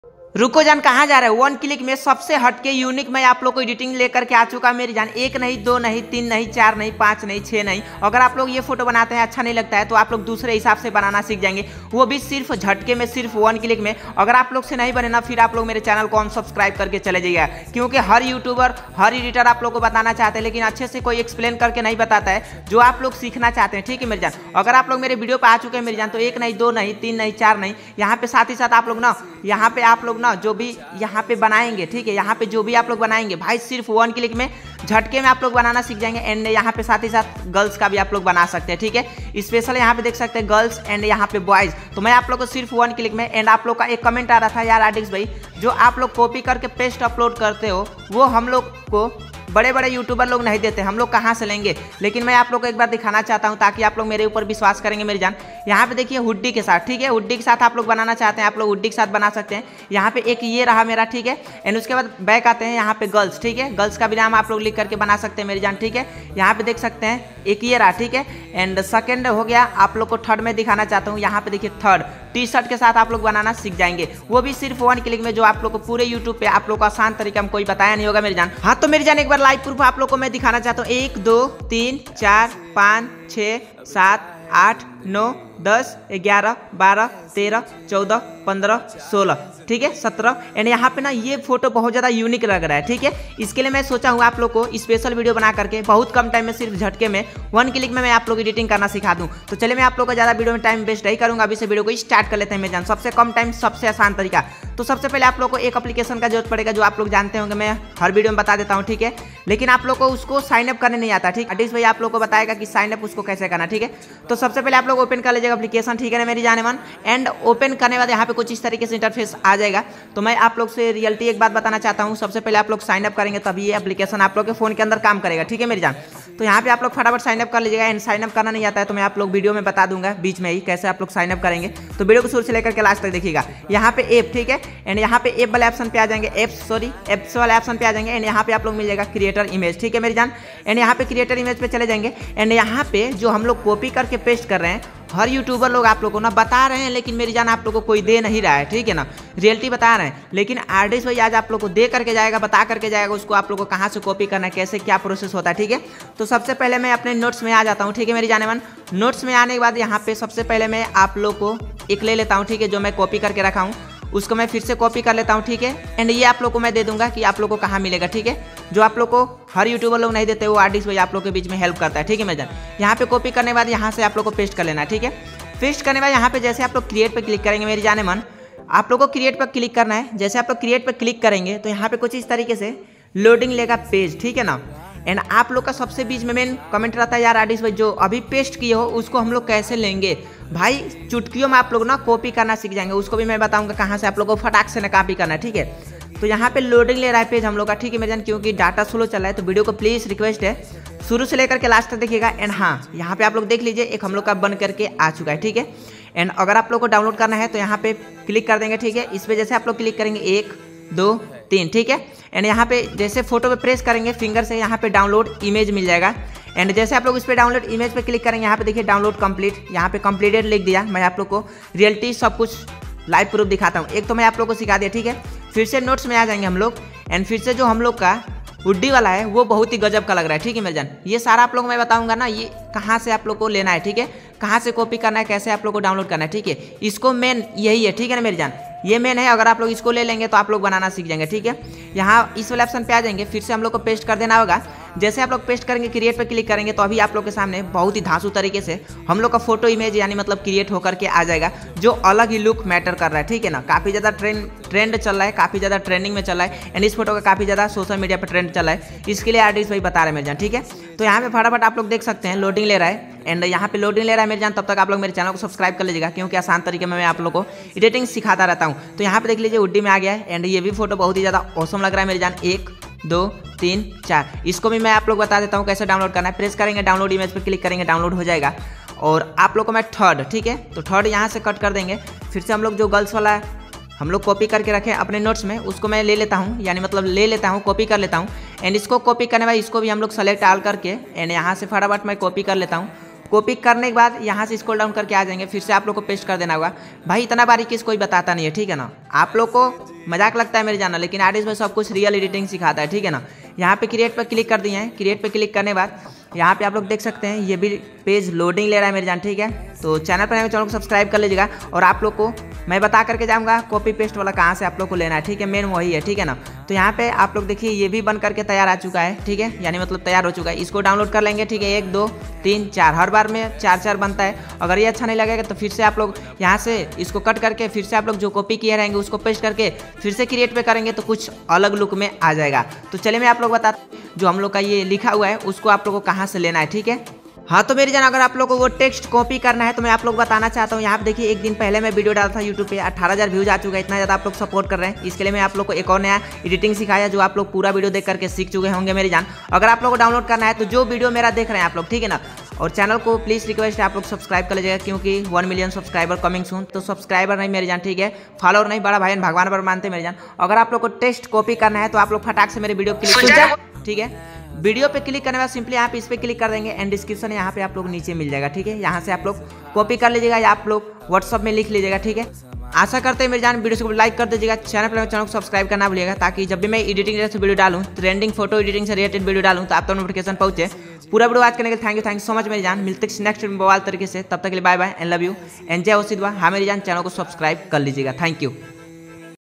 How To Create 3D Ai Wings Name Images | Bing ai viral editing | Bing image creator tutorial FREE
[HOODIE] - " Create a 3D illusion for a profile picture where a 25 Year old cute boy in a black hoodie Sitting casually on a Wingback chair. Wearing sneakers, with black mask, and sunglasses, he looks ahead. The background features "RDX editor" in big and capital Yellow neon light fonts on the dark grey wall. There should not be his shadow, and there are wings to make it appear as if he is an angel.

(1) - Boy

[SHIRT] - "Create a 3D illusion for a profile picture where a 25 Year old cute boy in a white shirt Sitting casually on a Wingback chair. Wearing sneakers, he looks ahead. The background features "rdxeditor" in big and capital Orange neon light fonts on the dark grey wall. There should not be his shadow, and there are wings to make it appear as if he is an angel.."

(2) - Girls

" Create a 3D illusion for a profile picture where a 25 Year old cute girl in a black hoodie Sitting casually on a Wingback chair. Wearing sneakers, with black mask, and sunglasses, he looks ahead. The background features "SONA" in big and capital Yellow neon light fonts on the dark grey wall. There should not be his shadow, and there are wings to make it appear as if he is an angel